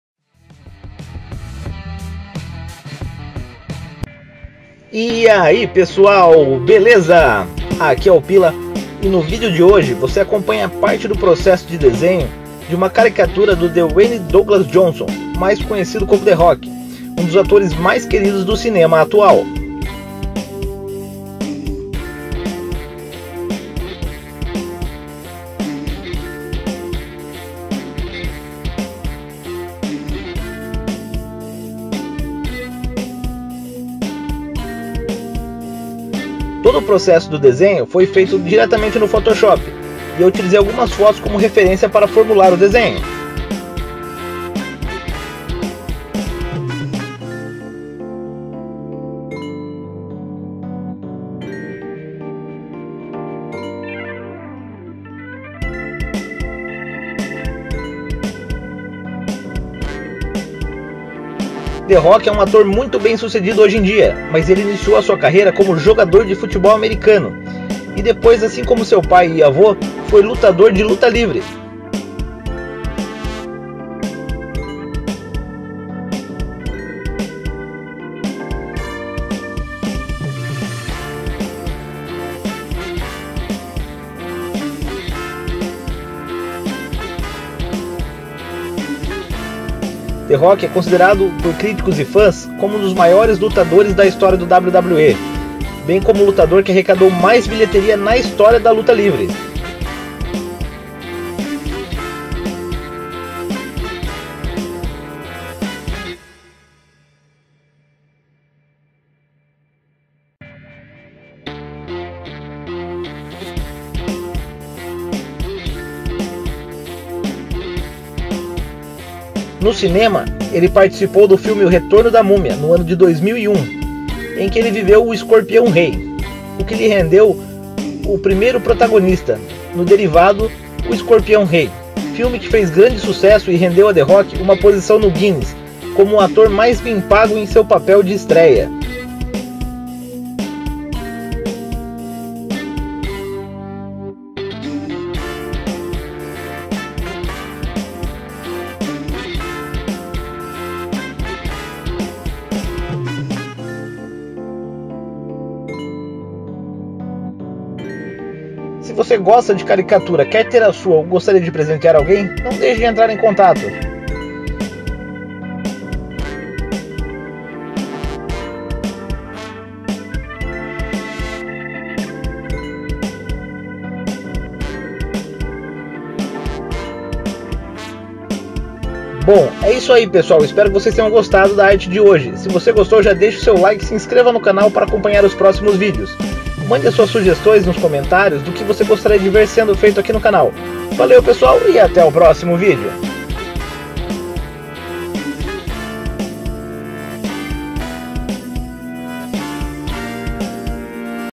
Caricatura do The Rock
Caricatura do ator Dwayne Douglas Johnson mais conhecido como The Rock, um dos atores mais queridos do cinema atual.Todo o processo desse desenho foi feito no Photoshop .